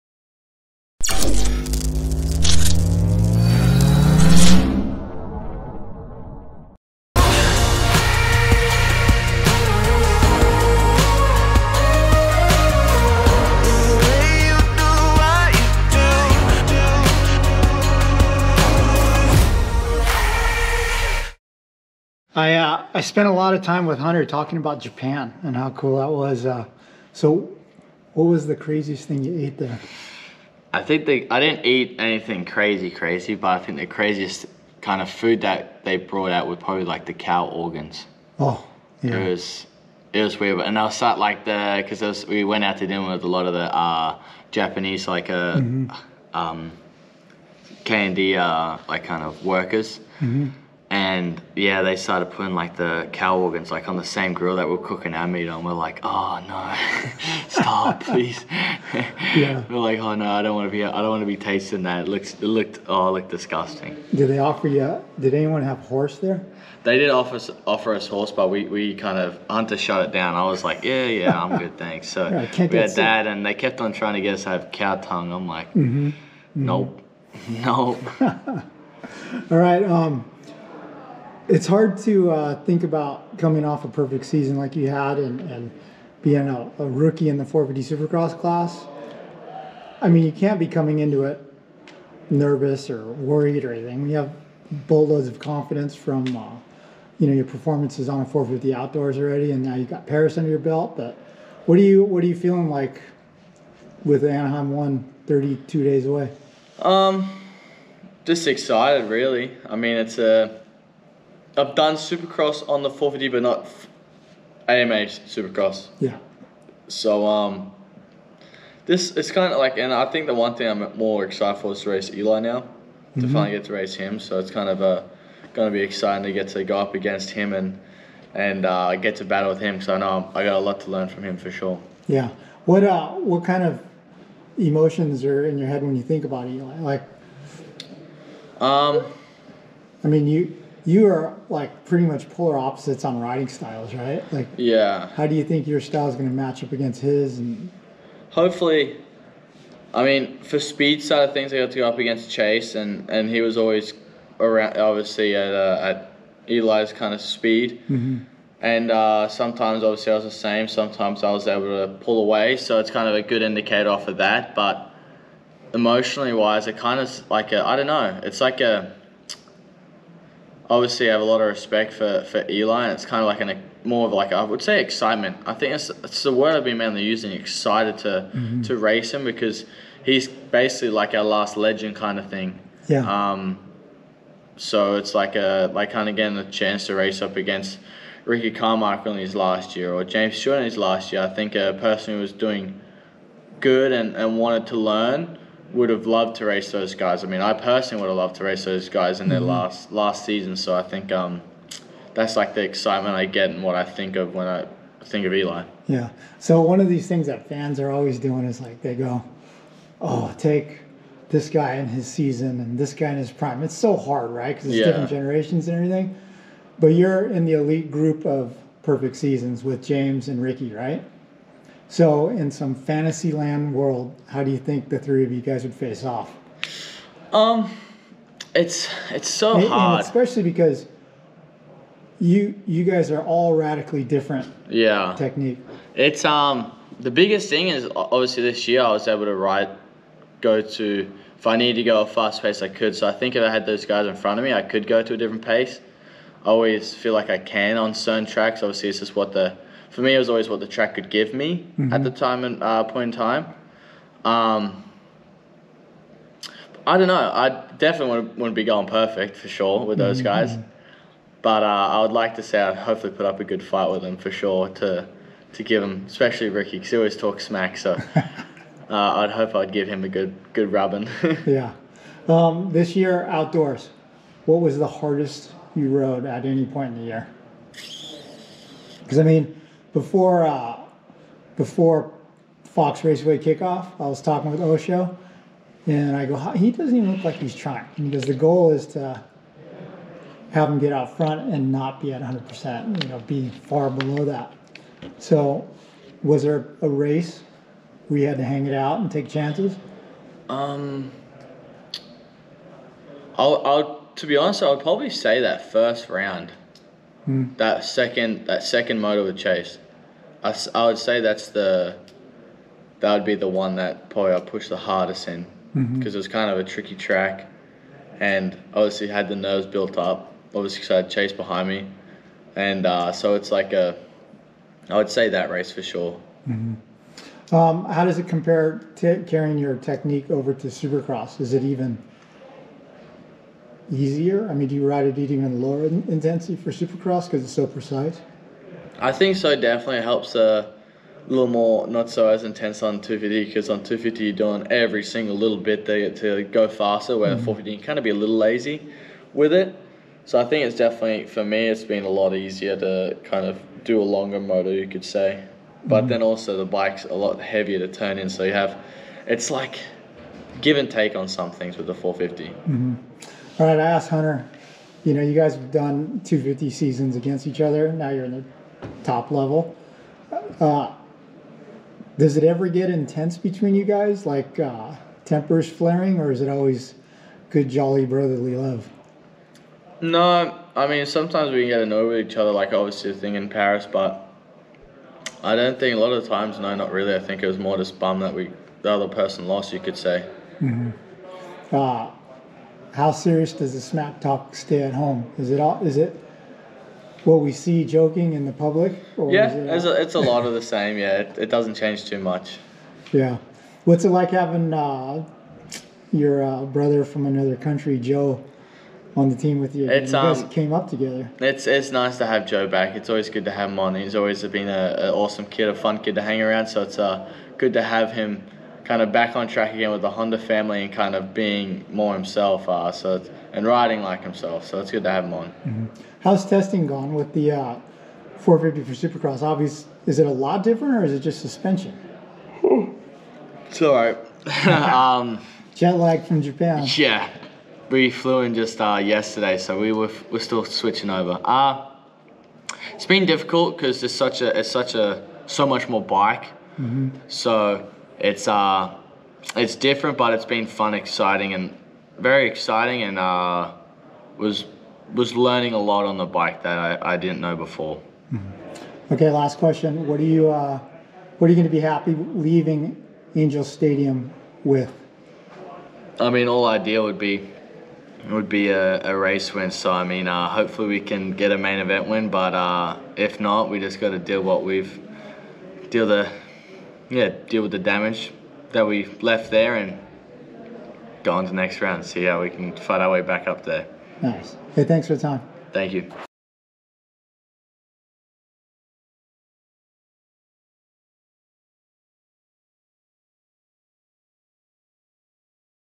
Jett Lawrence Has His Eyes Set on Eli Tomac!
Honda HRC invited members of the motocross media to the brand's US headquarters in Torrance, California, to see the new race bikes and interact with the racers. We spoke with Jett Lawrence about the expectations that his perfect motocross season brings to his rookie 450 Supercross series that kicks off in a mere 32 days. We asked him what he was most excited about heading into Anaheim One..."I am so excited to race against Eli." Press play and hear the rest!